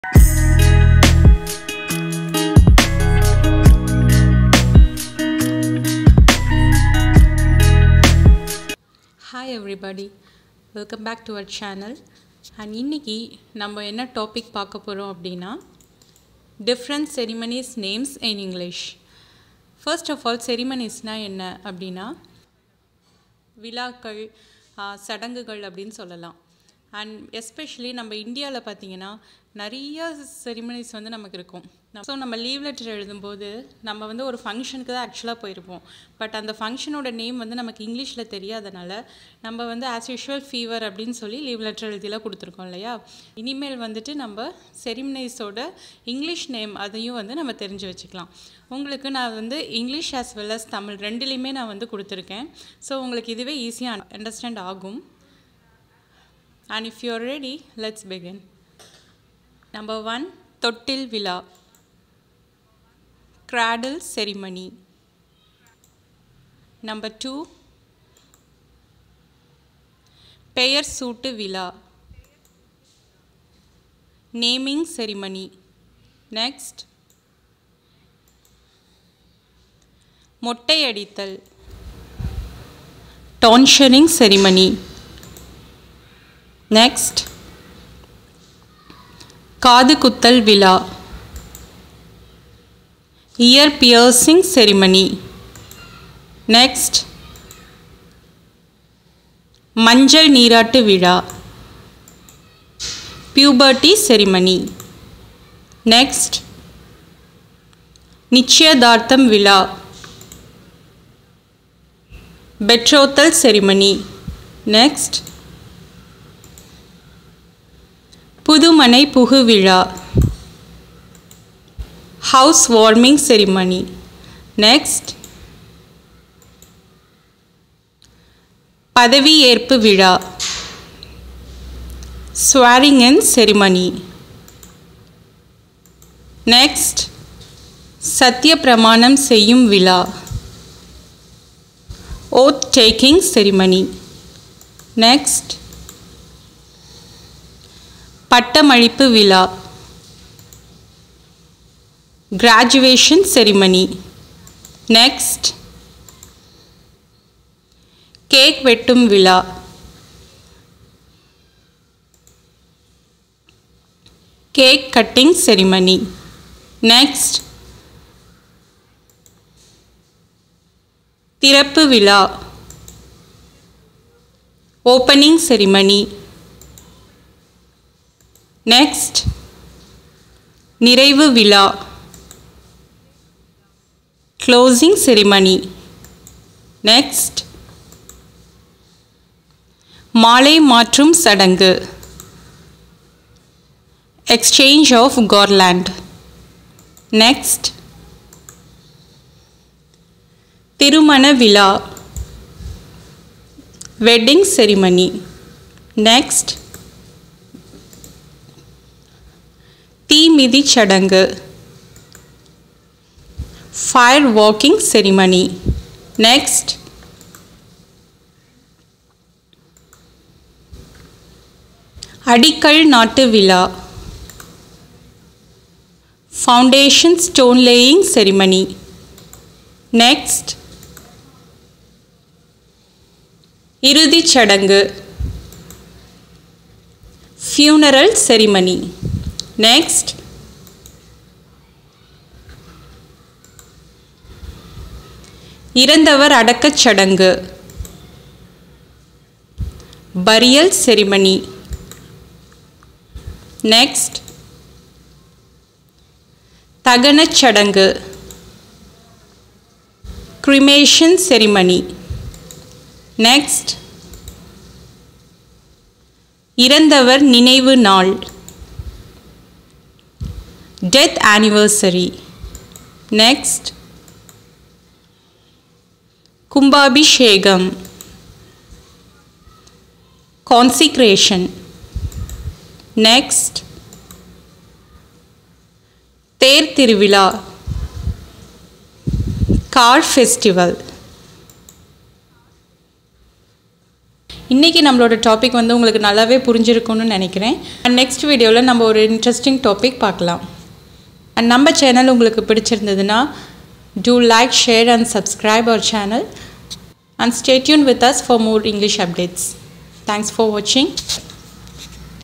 Learn different ceremonies names in English
How to say different ceremonies names in English
Know various ceremonies names through Tamil